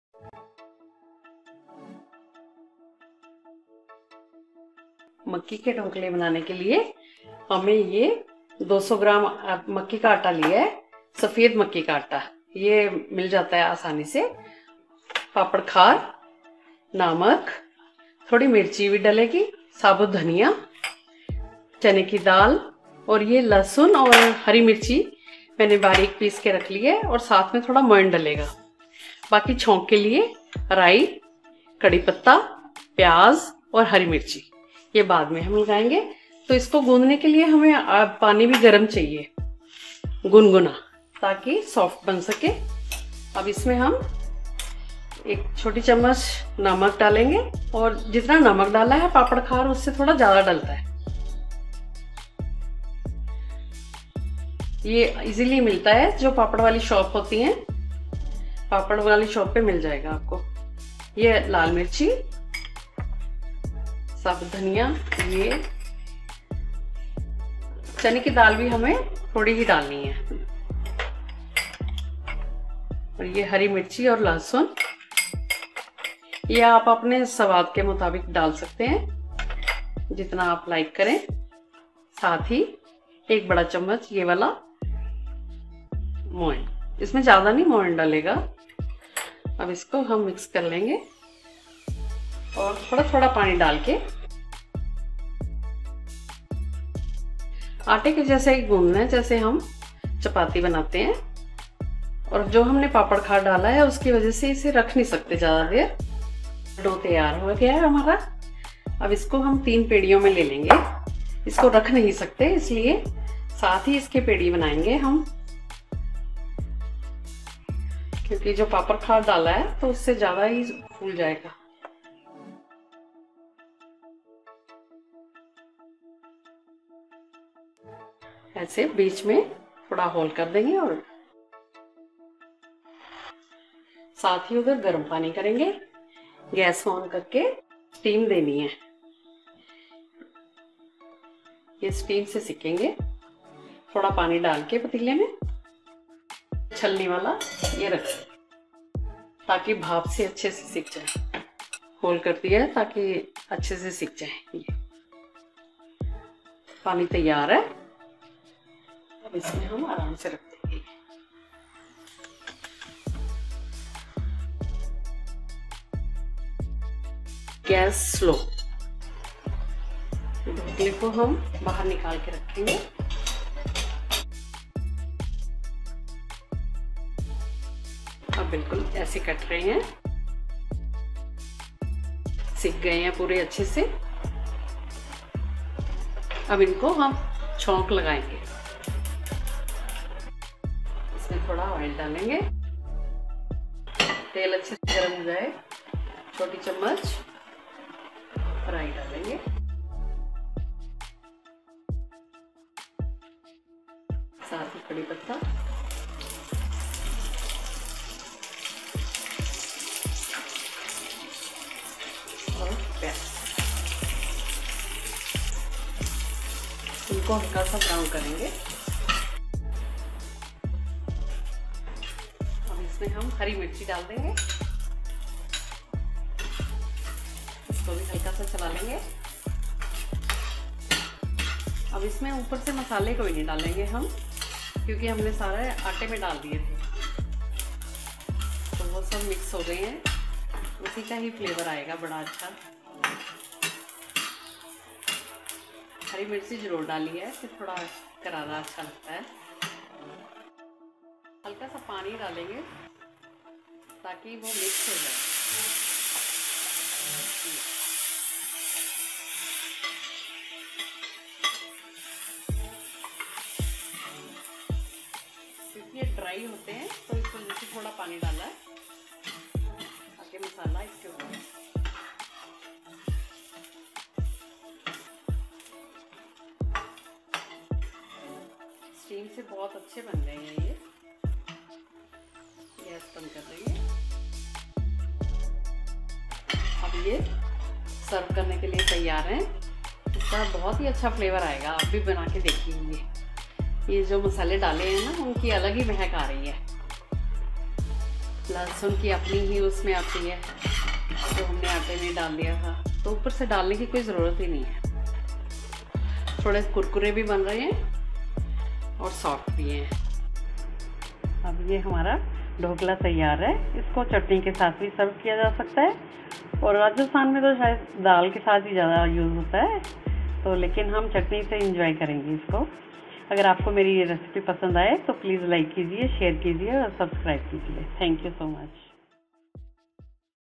Makki ka Dhokla recipe | राजस्थानी मक्की के आटे से ढोकला | Cornmeal/Maize flour Dhokla Recipe
Enjoy this quick and delicious recipe from the heart of Rajasthan! Makki ka Dhokla is one of the most common snacks item readily enjoyed in almost all households in Rajasthan.

Its best to go with any Daal or you can just enjoy on its own with some Pudina chutney as well.